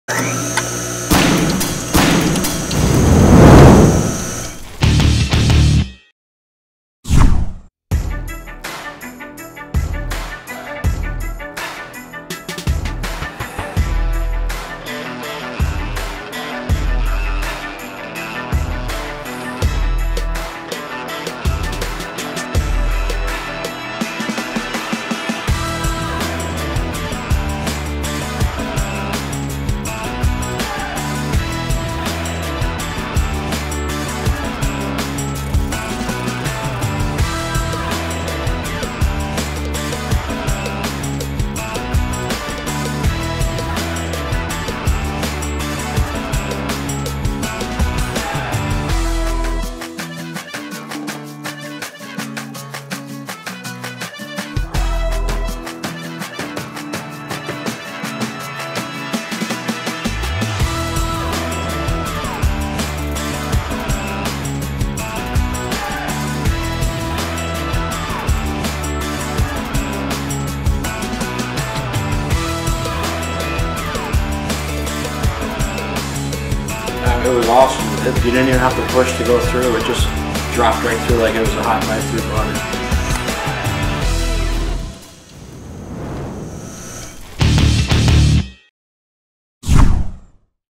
TIGER UltraCut Wheel
Weiler’s NEW Tiger UltraCut wheels deliver unmatched cutting performance and toughness with a TRUE 1 millimeter thickness. Best on thin sheet metal, tubes, profiles and small cross-section rods.